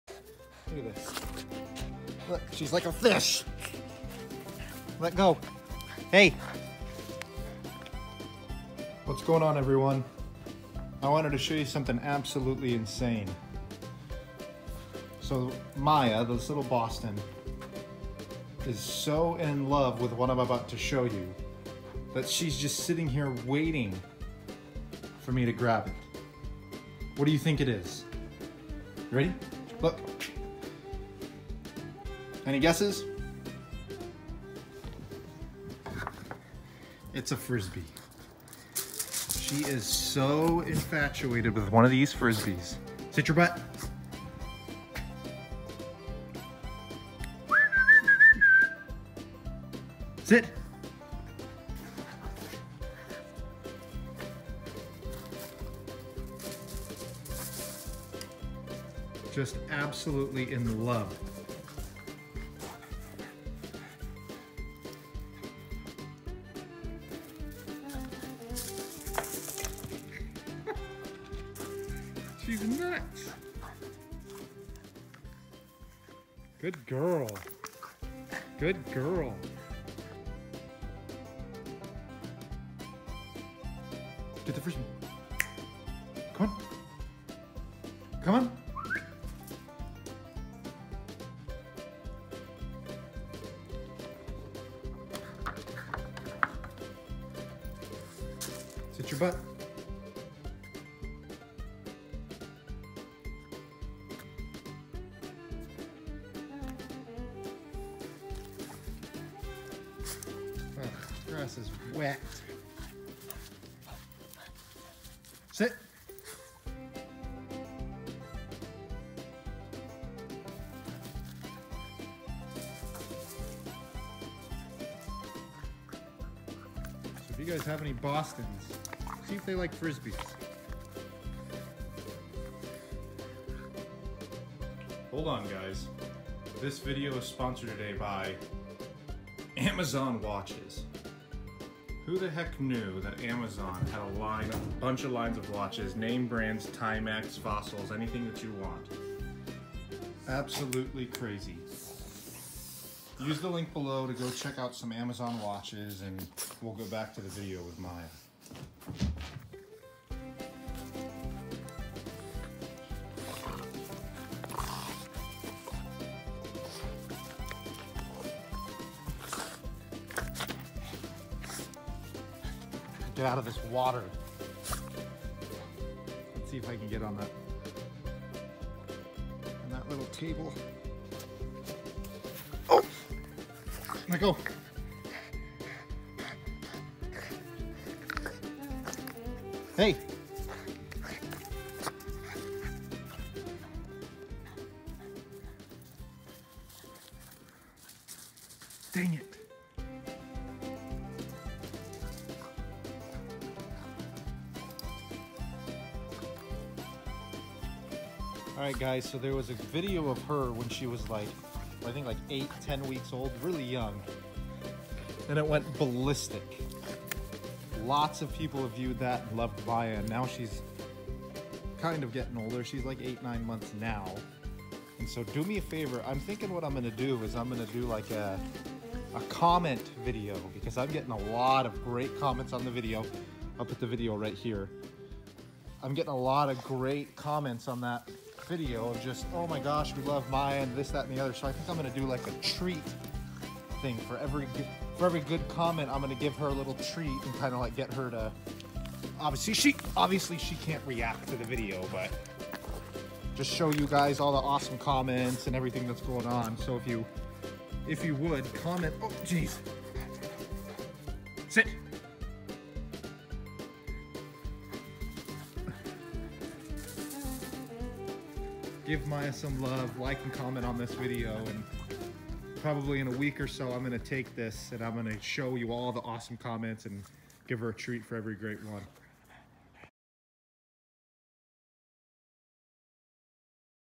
Boston Terrier puppy goes CRAZY for a frisbee!
In this video, Maya is a 9 month old Boston Terrier puppy. And she goes crazy for Frisbees! This puppy loves to play with pretty much anything but she loves frisbees! She is a super cute dog and love my boston terrier!

Let me know if you have a boston or what kind of dog you have! Or is you have any questions about her!

***Amazon Watches***
Amazon has a whole line of watches, name brands, great prices. Go check them out with my link!

Copyrights, all the music came from Imovies free library